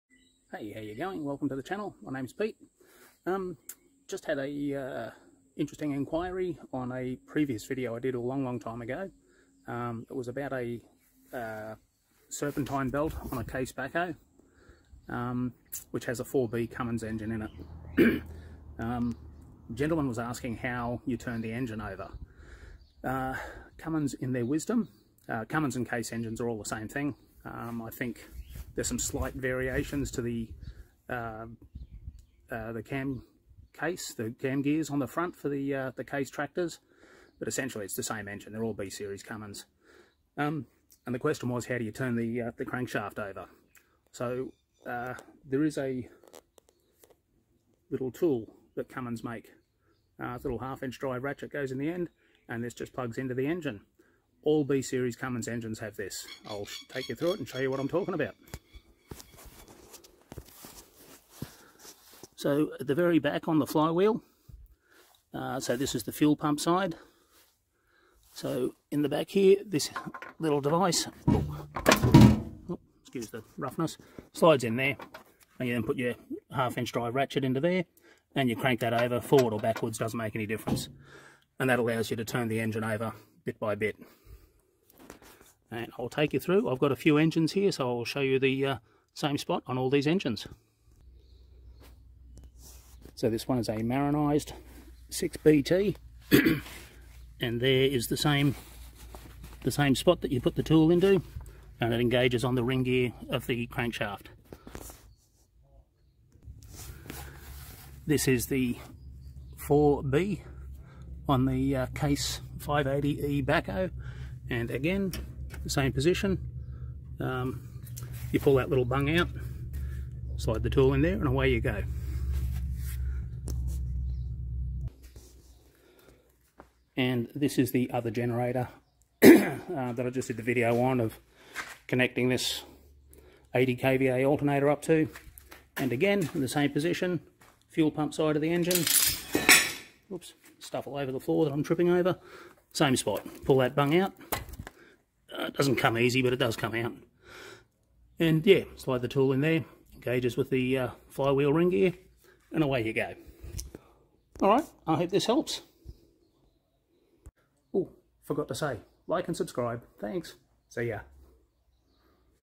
Cummins/Case 'B series' turnover tool
A quick explanation of how to 'inch over' a 'B series' Cummins or Case engine.